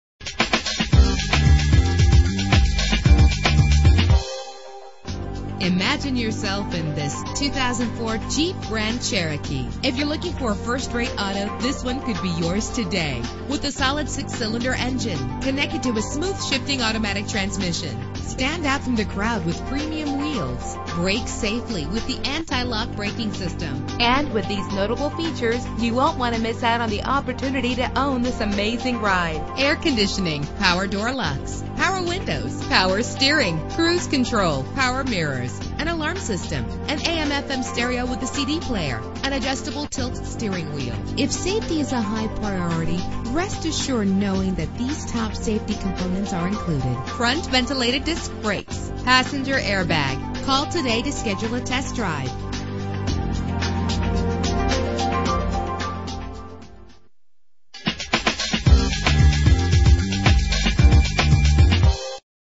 2004 Jeep Grand Cherokee - St. George UT
Year: 2004
Make: Jeep
Model: Grand Cherokee
Trim: Laredo
Engine: 4.0 liter inline 6 cylinder 12 valve
Transmission: Automatic
Color: Midnight Blue Metallic
Mileage: 103228
Address:
1629 S Convention Center Dr